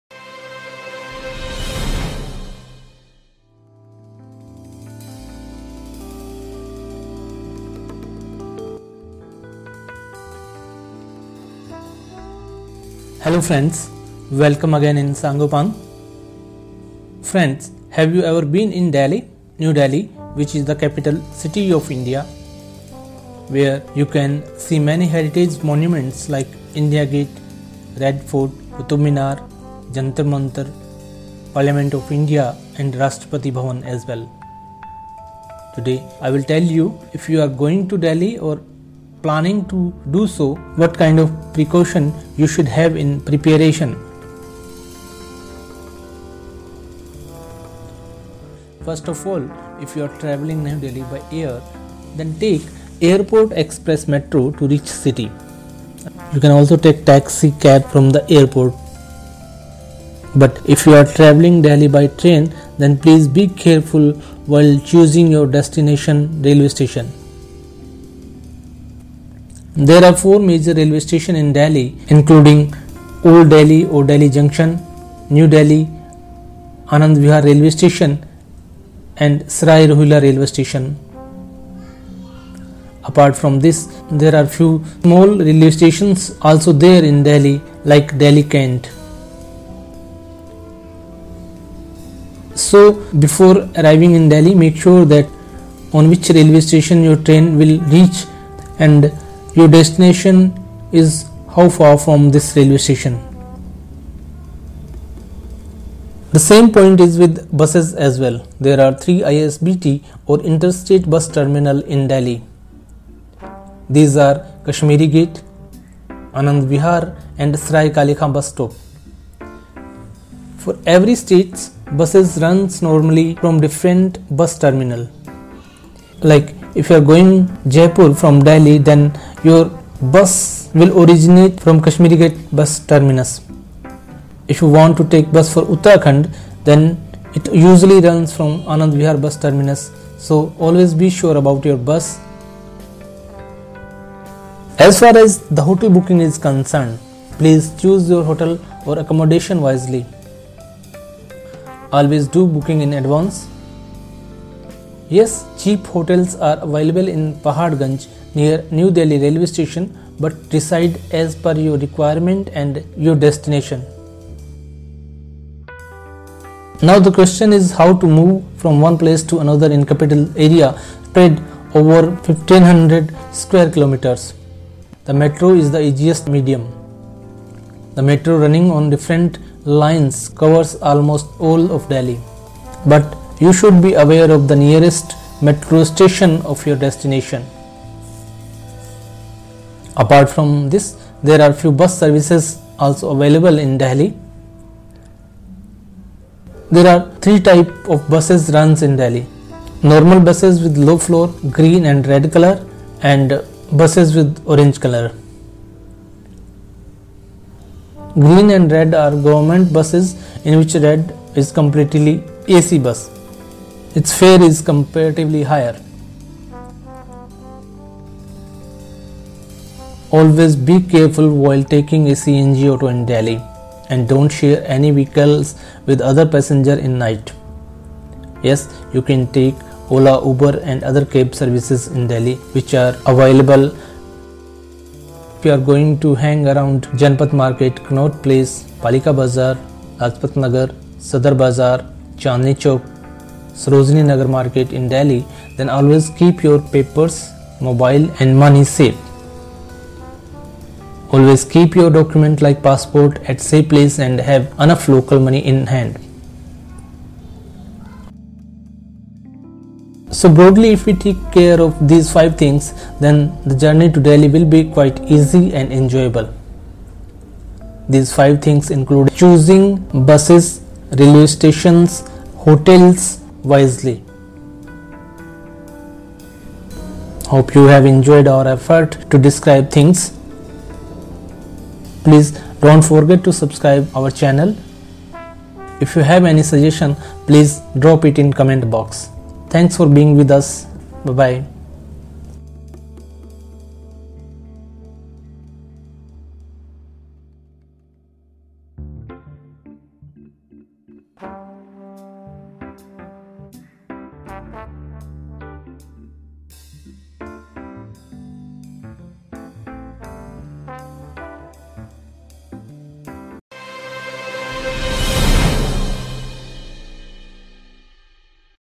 Tips To Travel Delhi | sangopang
Delhi or New Delhi, is the capital city of India.  Where you can see many heritage monuments like India Gate, Red Fort, Qutub Minar, Jantar Mantar, Parliament of India and Rashtrapati Bhavan  etc. Please watch the video to know more about the city and precaution you should have while travelling this city.